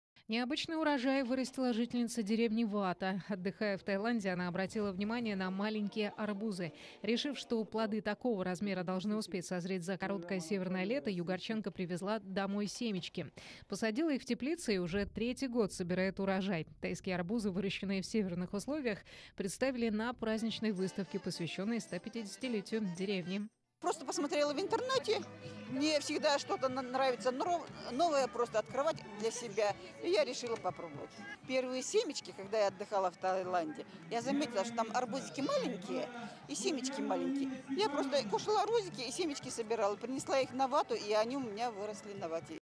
Югорчанка в теплице вырастила тайские арбузы
Необычный урожай вырастила жительница деревни Вата. Отдыхая в Таиланде, она обратила внимание на маленькие арбузы.